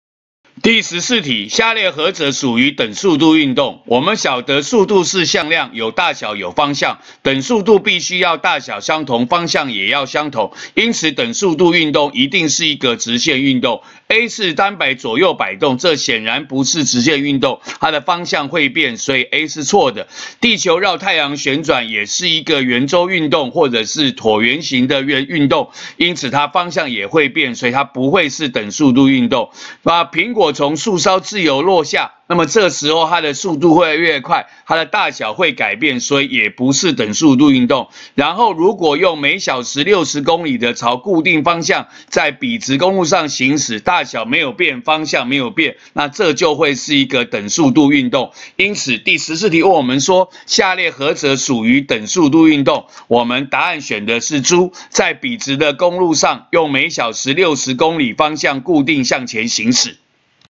106學年上9年級理化一段第14題
問何者屬於等速度運動。